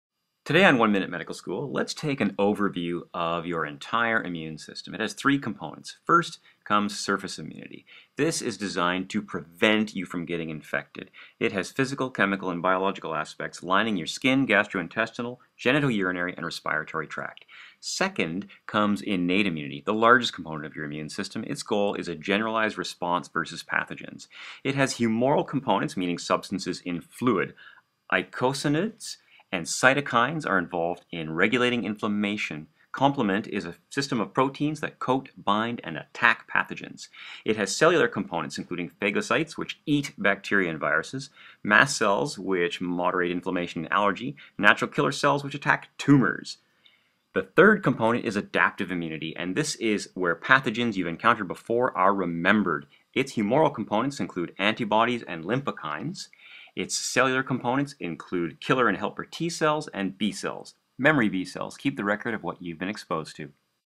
Immune System Overview - One Minute Medical School
Dr Rob reviews the entire immune system in 60 seconds.